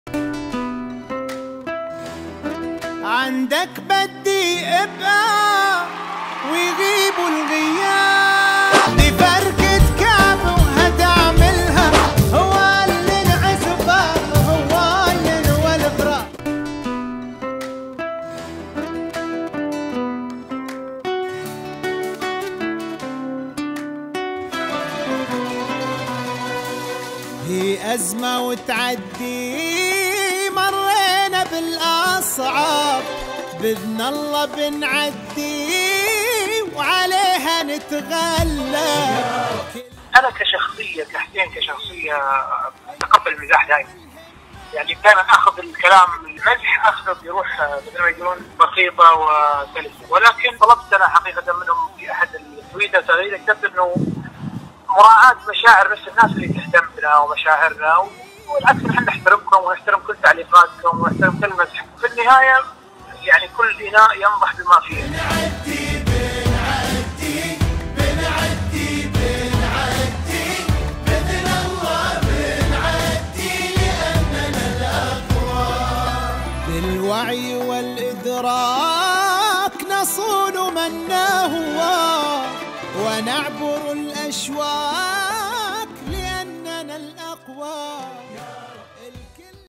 حسين الجسمي يواجه التنمر بالحب
حسين_الجسمي، الفنان المسكون بحب الإنسانية، يواجه التنمر بالحب والتسامح والصمت#